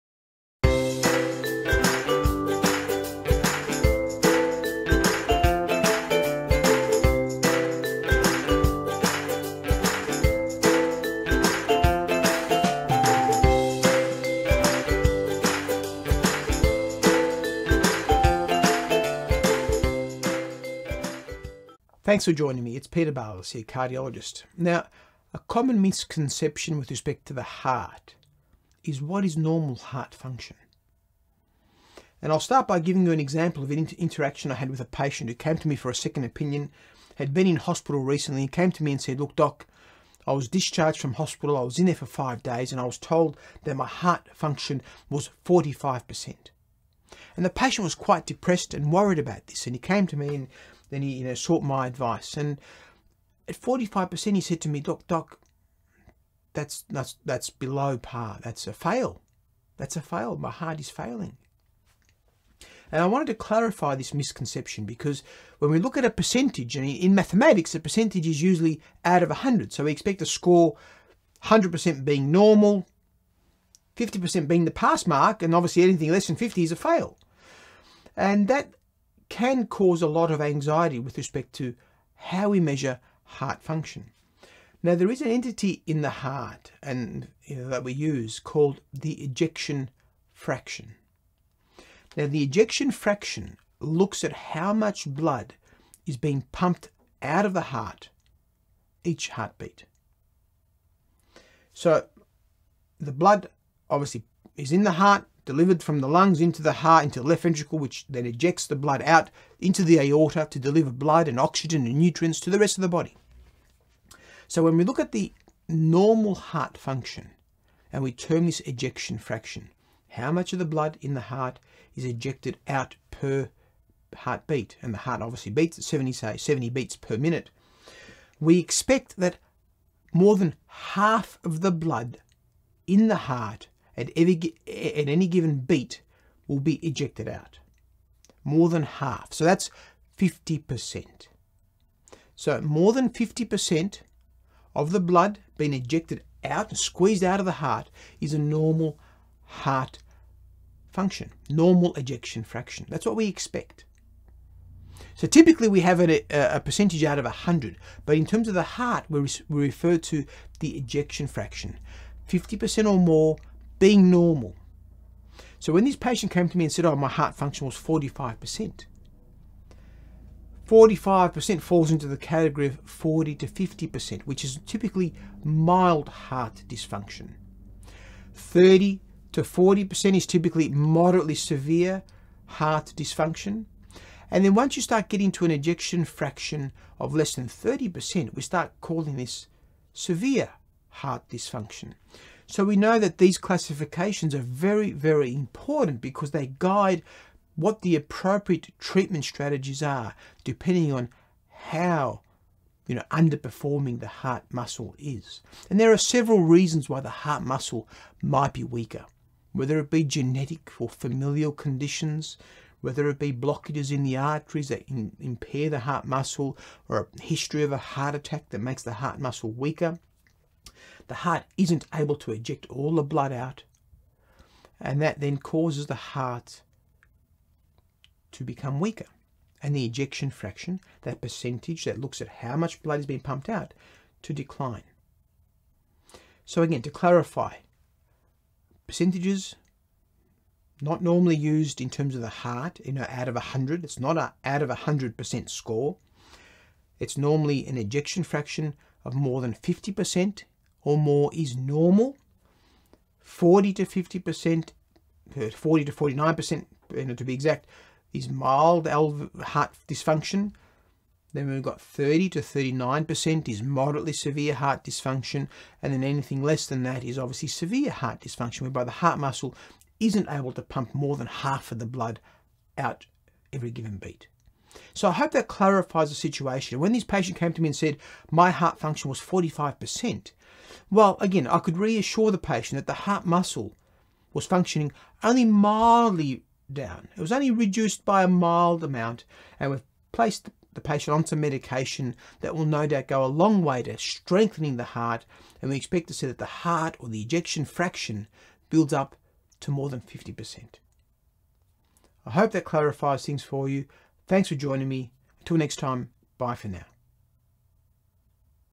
What is normal heart function?
In this brief video we go over what is considered 'normal' heart function and elaborate on a common misconception.